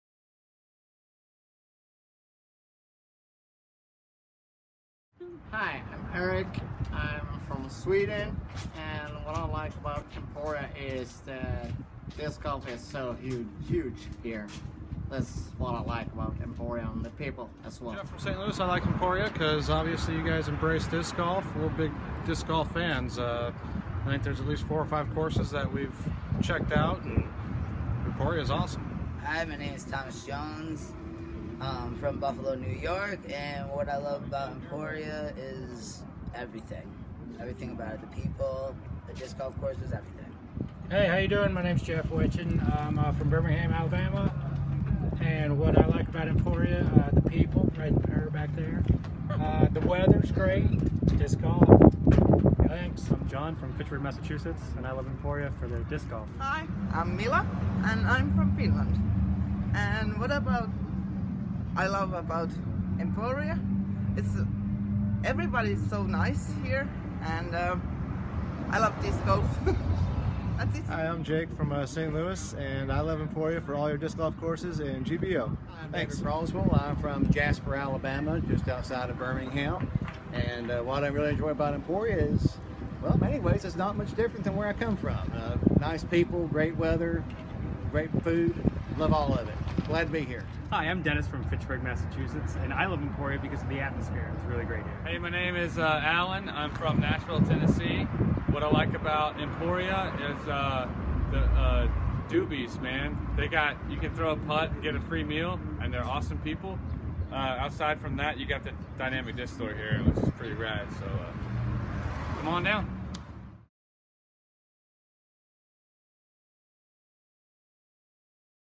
Disc Golfers at GBO Appreciate Emporia, Kansas
Disc golfers who come to Emporia, Kansas to play in the Glass Blown Open are appreciative of all things disc golf and the community.